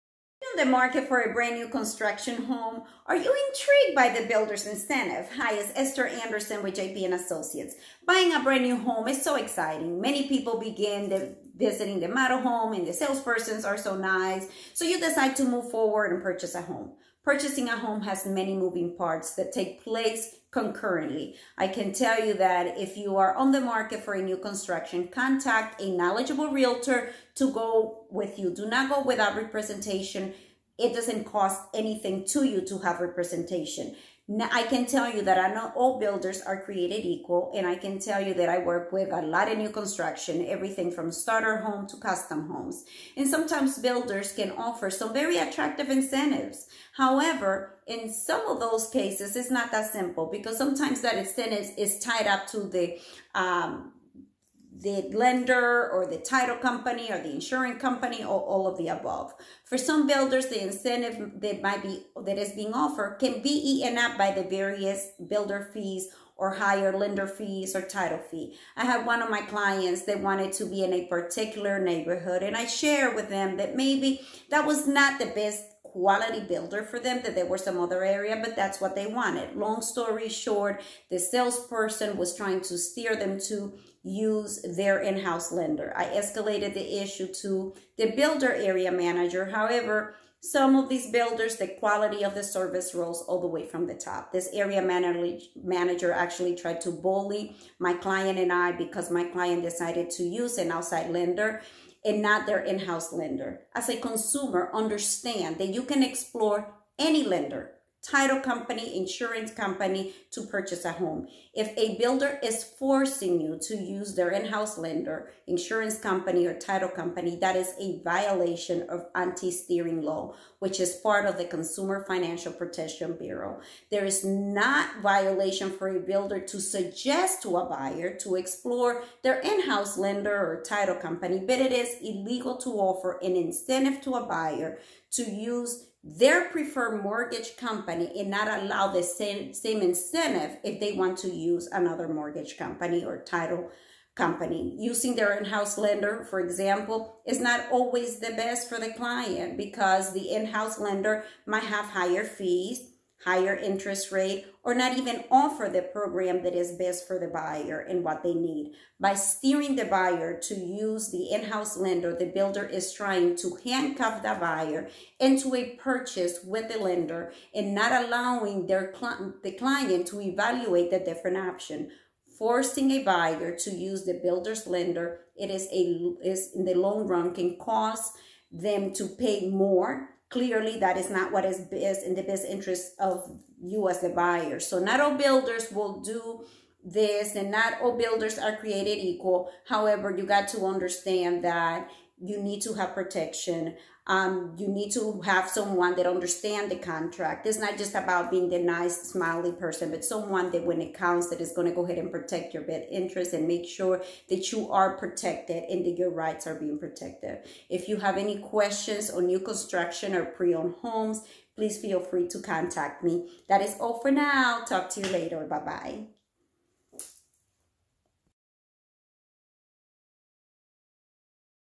Are you on the market for a brand new construction home ?
Today insider tip. This violates the Consumer Protection Bureau, a violation of AfBA exception to RESPA. The builder and their team do not work for the buyer; they work for themselves.  Make sure that you have representation when you are purchasing new construction, and your rights and best interest are ALWAYS protected. No, you do not save ANY money by not using a Realtor; most builders pay for the Realtor broker fee. Why not have an advocate on your side, protecting you during one of the most significant purchases?